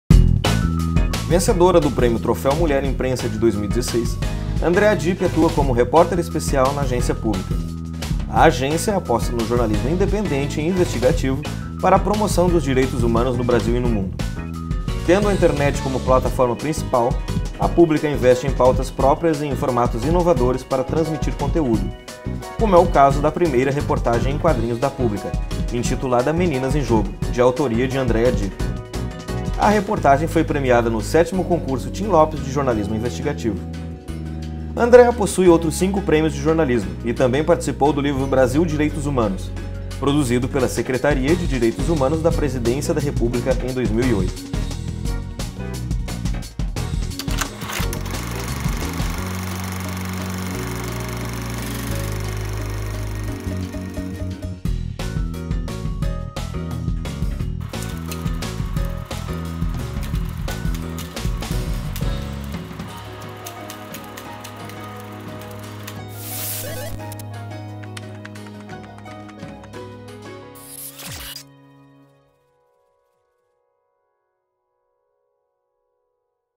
Andrea Dip no Narrativas em Movimento
Andrea Dip é repórter especial da Agência Pública em São Paulo. Andrea vem até Santa Maria - RS para participar do Colóquio 100/20: jornalismo na era da internet, no dia 12 de maio, às 19h no Theatro Treze de Maio.